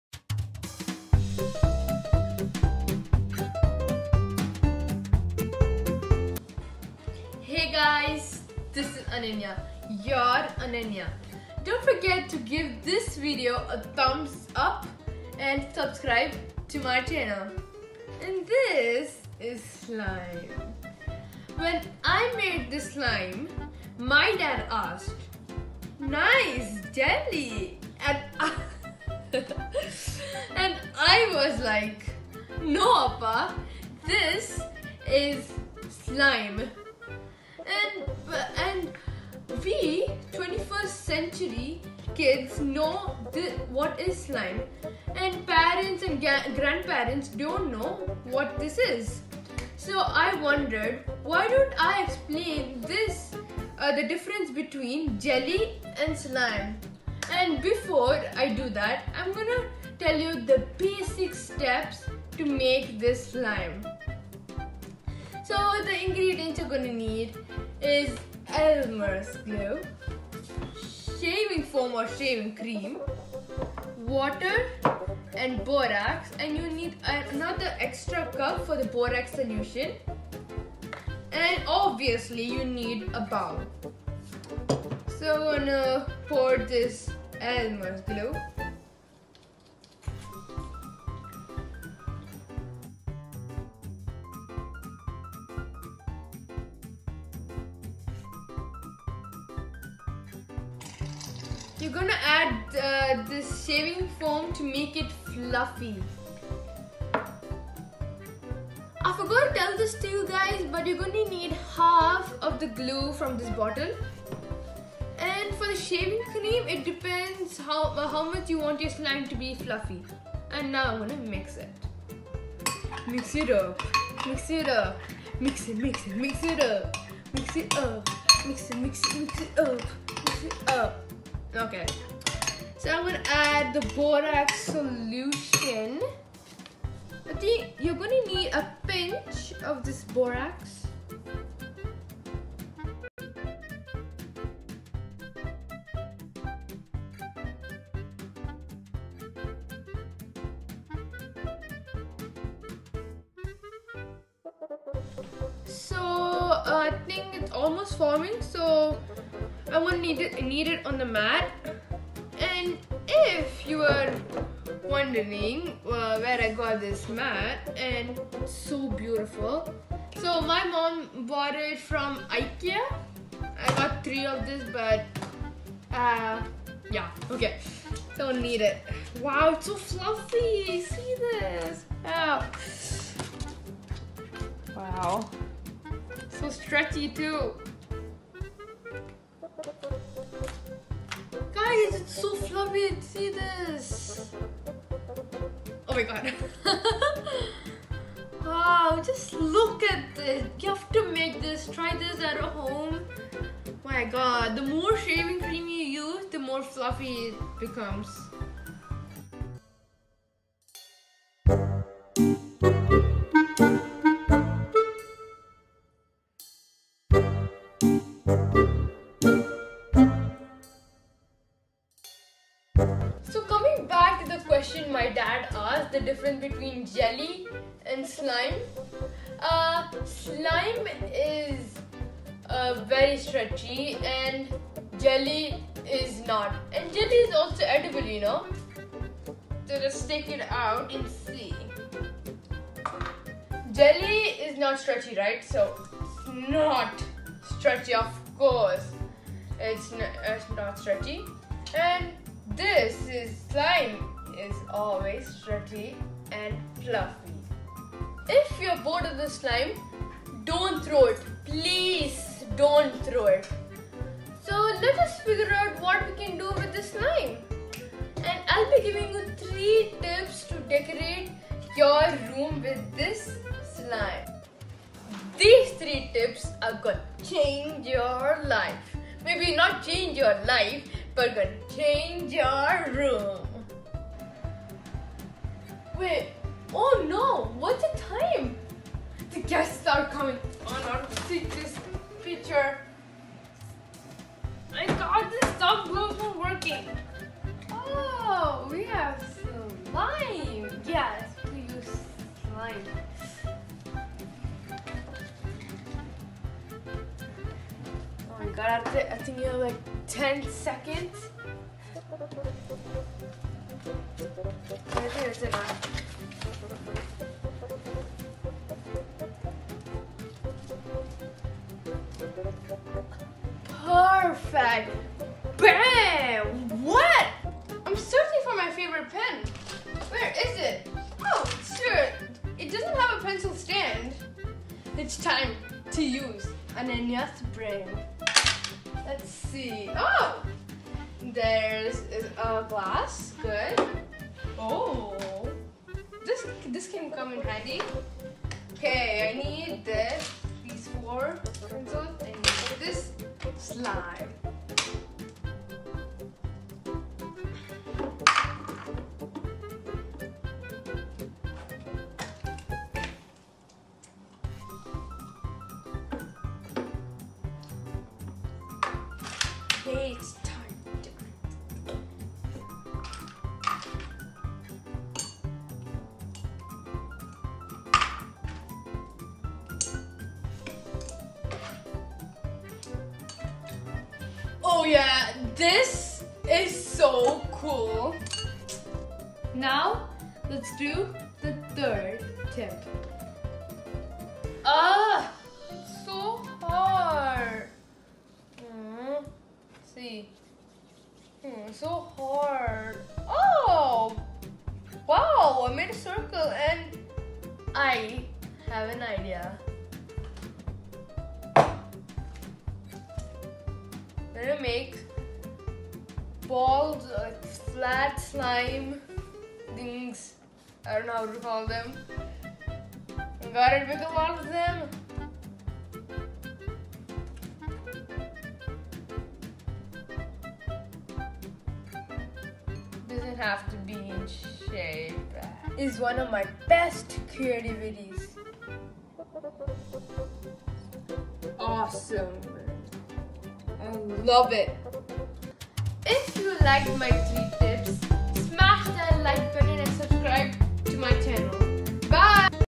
SLIME IDEAS
This video gives you 3 simple ways to use slime.